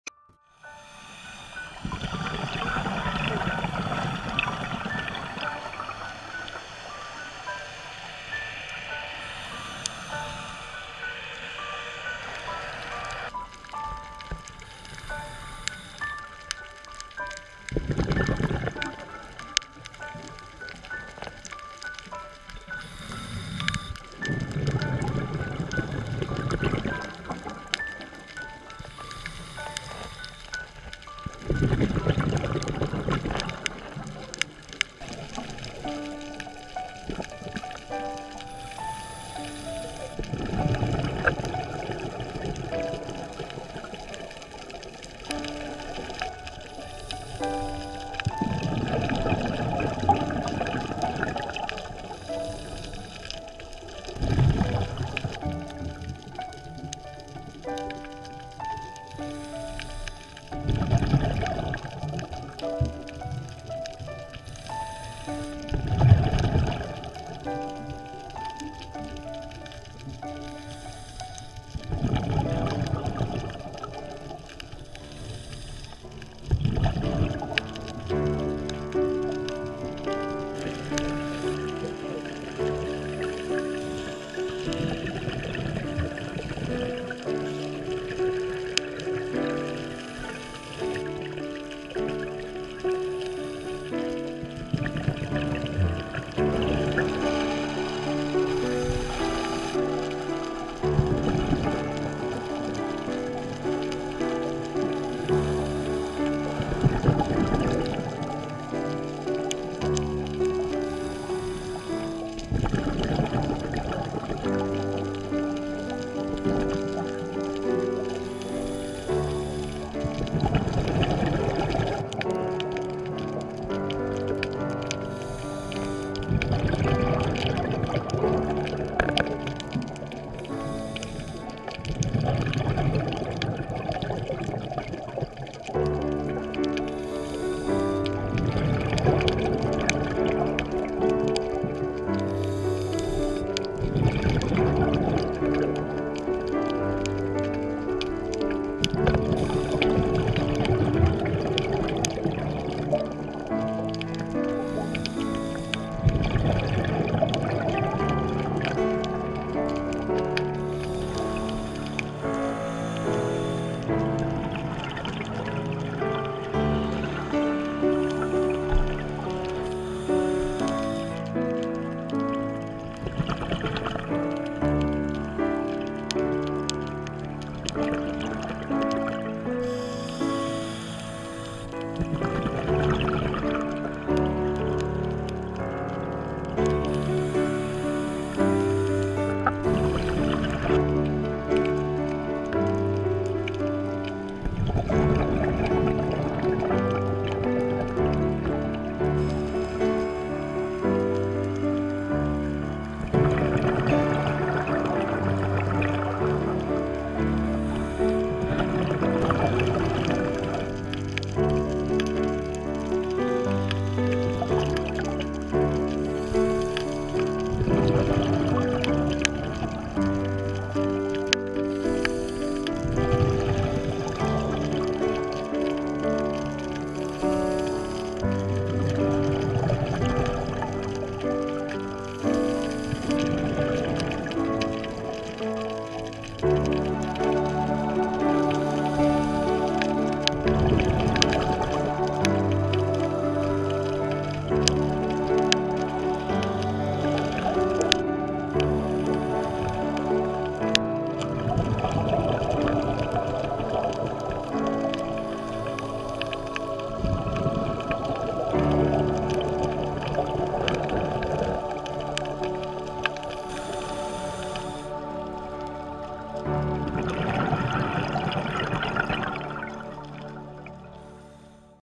Tremiti Islands - Macro Footage
Un breve filmato macro, girato alle Isole Tremiti.
A short macro footage, shooted in Tremiti Island, Italy.

Immersioni fatte con gli amici del Tremiti Diving Center.
Diving with the friends of Tremiti Diving Center